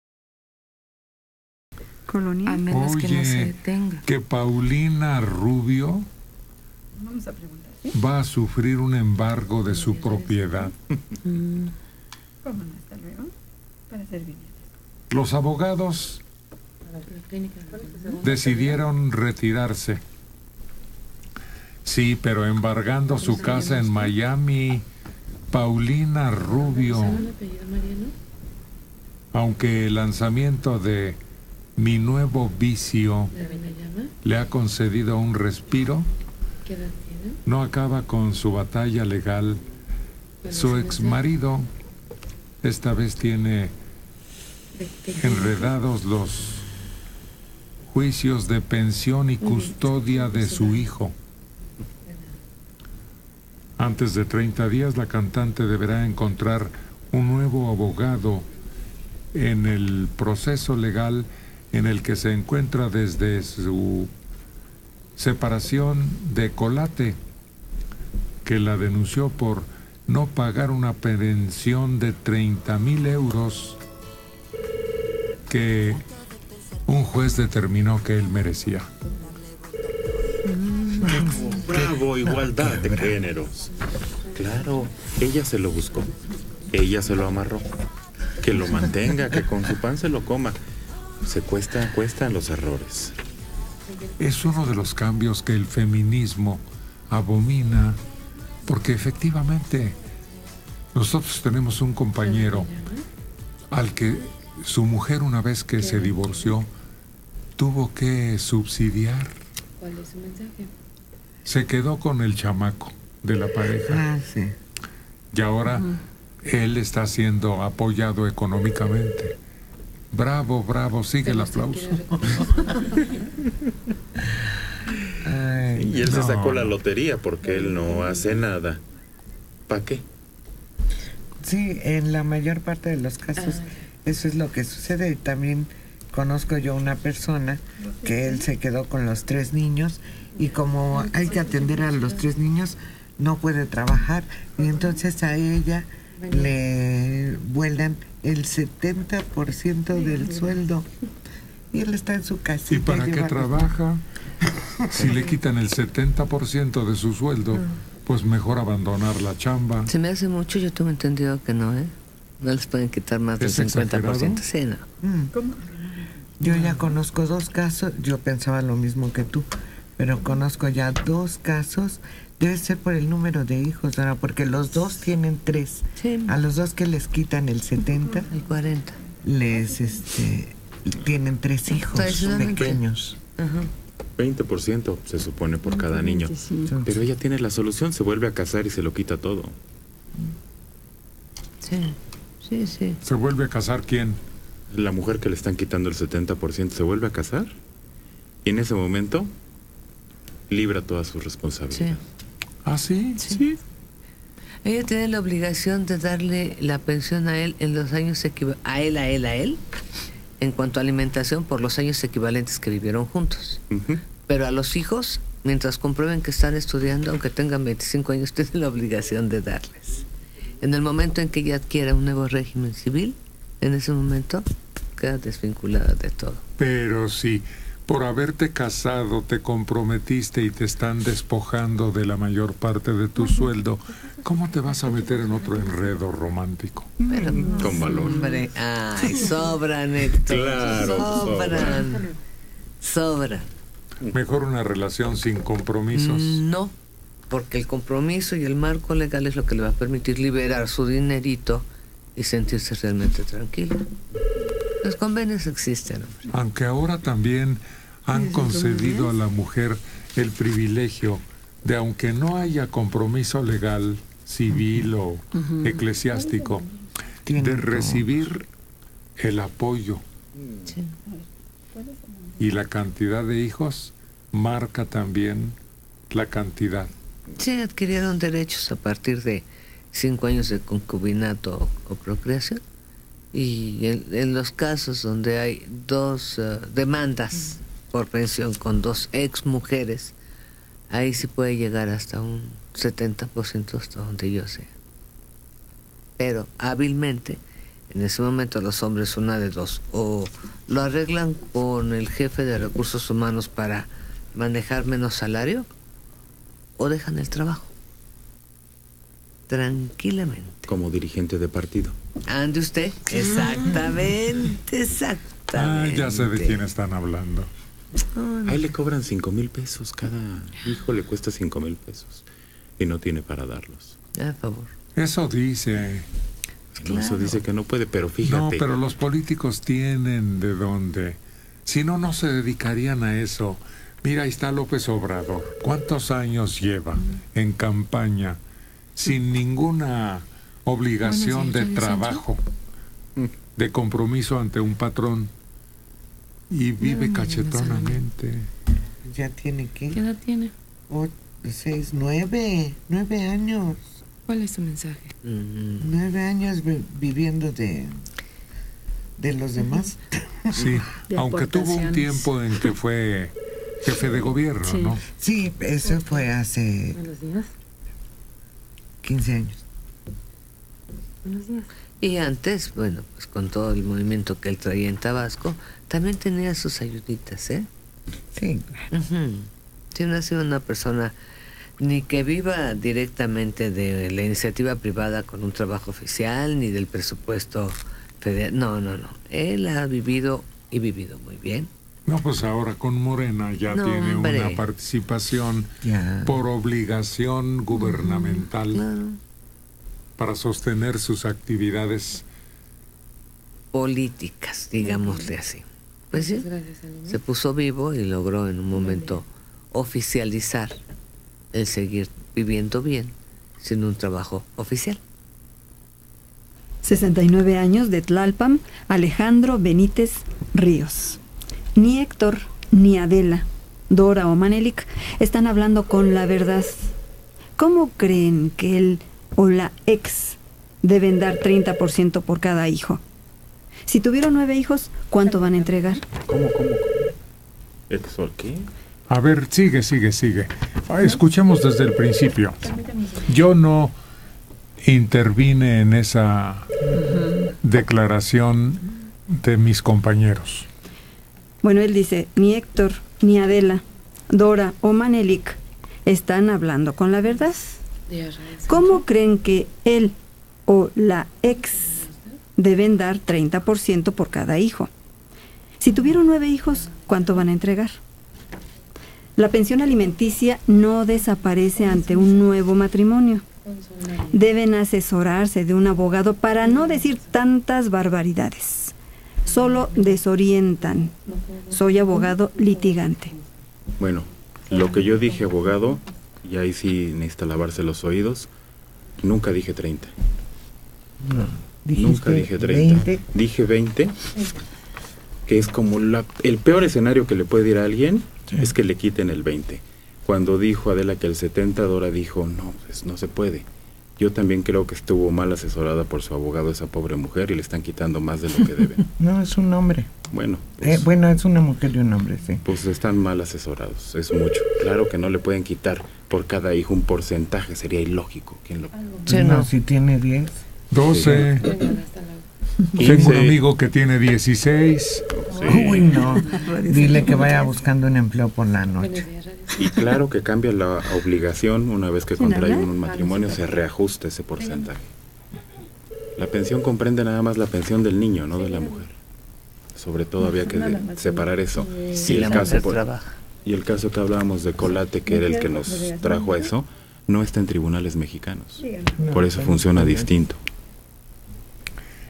Paulina Rubio podría ser embargada - Martínez Serrano
paulina rubio sera embargada pleitos con su ex esposo colatte